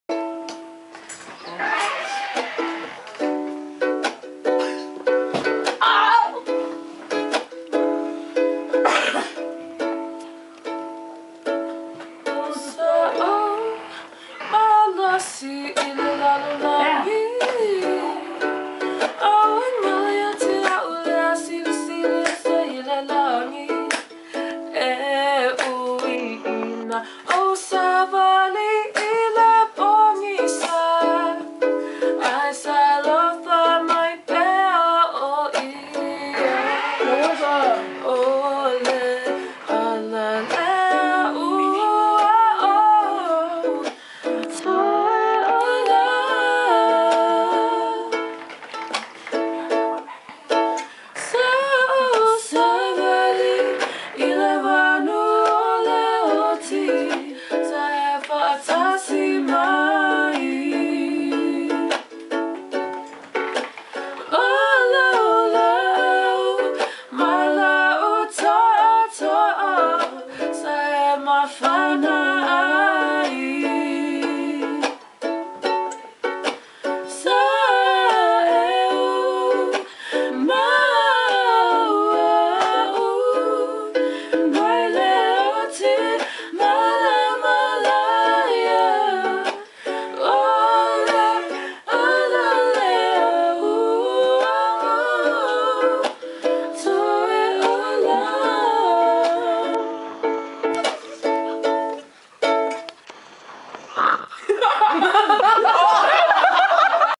Sa Ou Malosi
Taiapisi&Aisa Logotala messing around on a typical Sunday..fambizz  lol